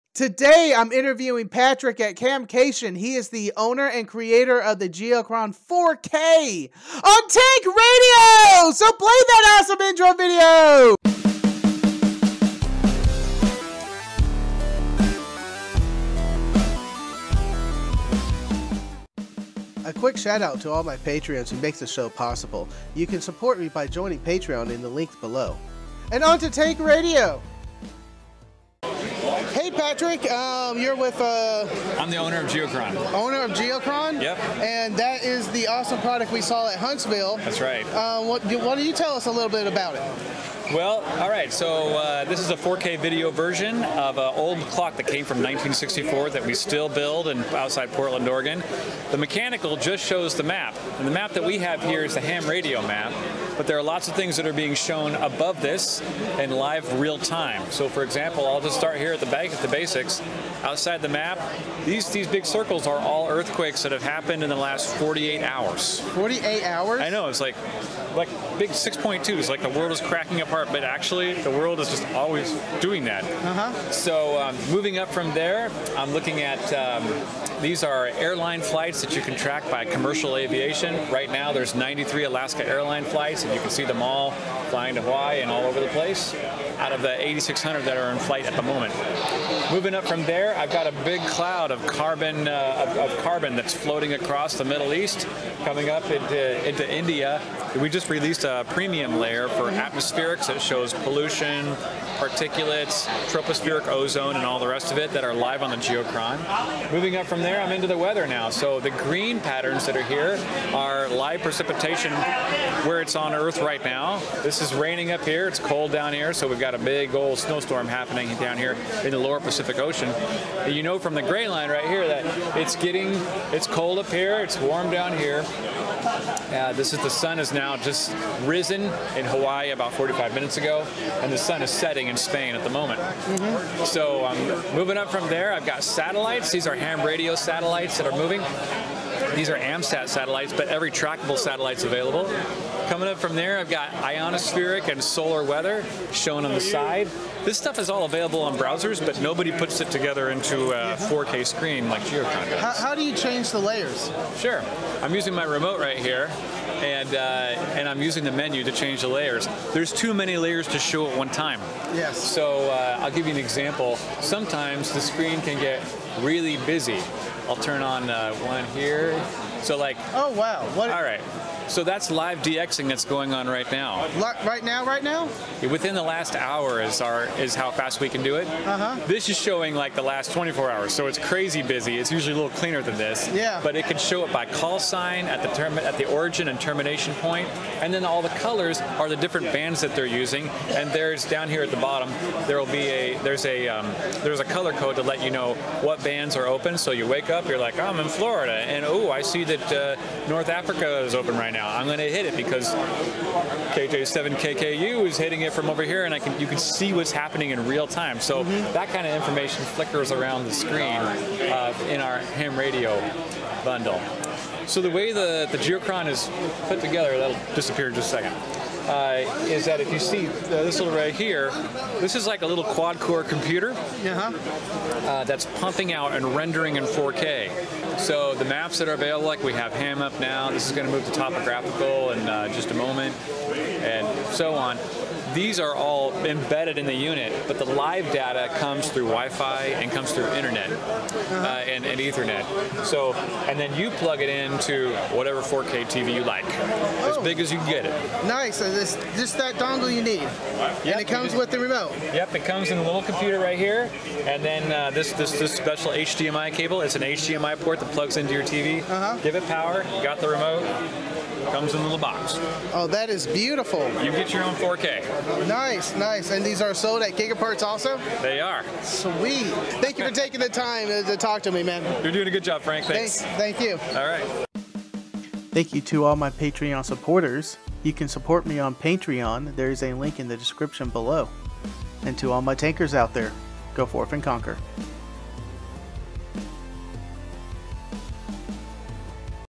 Geochron Atlas 4k features
Patrick the Founder and Creator of Geochron will show us around the Geochron 4K Altas and some features that are for Ham Radio Operators.